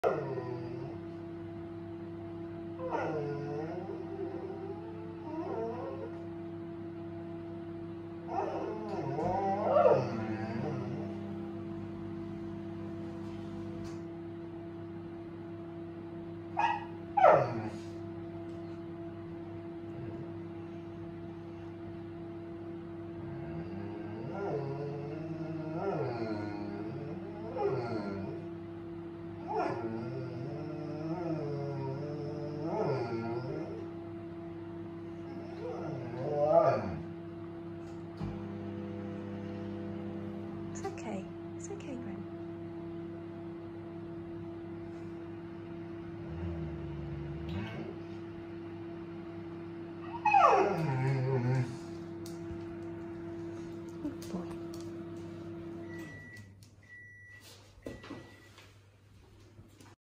Grenson airedale really wants his dinner...
Grenson can be pretty vocal when he knows what he wants and isn’t getting it fast enough!